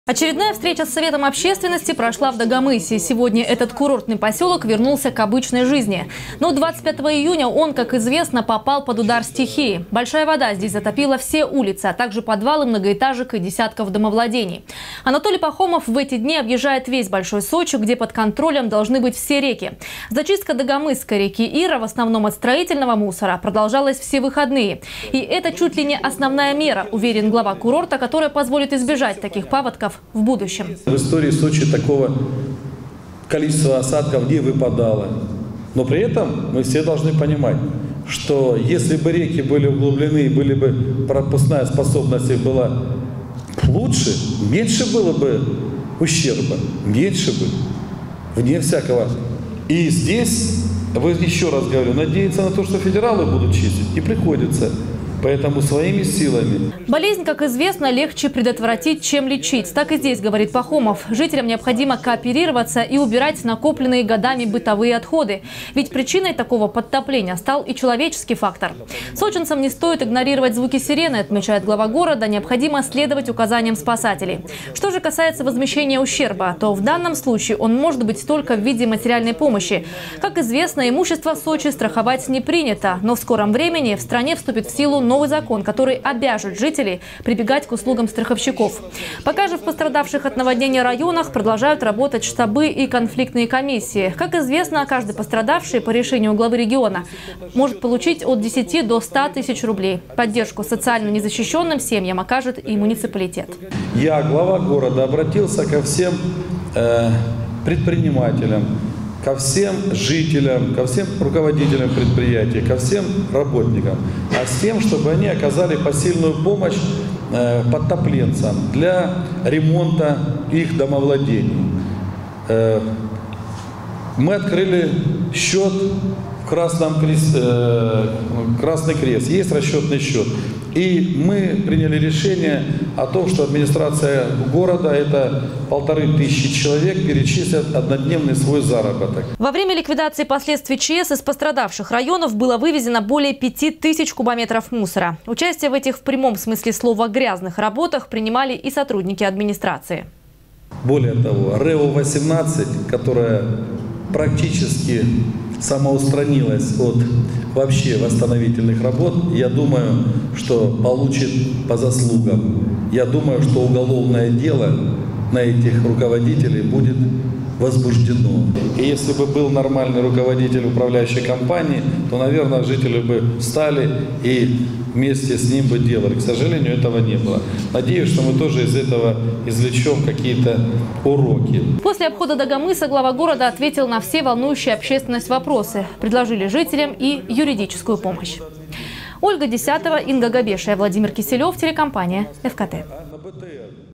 Пахомов провел встречу с общественностью Дагомыса Новости Сочи Эфкате
Однодневный заработок чиновников в помощь подтопленцам. Пострадавшим от стихии сочинцам продолжают оказывать материальную помощь. Работы же по устранению последствий наводнения на сегодняшний день завершены. Руководство города продолжает анализировать работу по ликвидации ЧС.

Очередная встреча с советом общественности прошла в Дагомысе. Сегодня этот курортный посёлок вернулся к обычной жизни. Но 25 июня он, как известно, попал под удар стихии. Большая вода здесь затопила все улицы, а также подвалы многоэтажек и десятков домов. Анатолий Пахомов в эти дни объезжает весь Большой Сочи, где под контролем должны быть все реки. Зачистка дагомысской реки Ира, в основном от строительного мусора, продолжалась все выходные. И это чуть ли не основная мера, уверен мэр, которая позволит избежать таких паводков в будущем.

Сочи. Новости.
Телеканал Эфкате.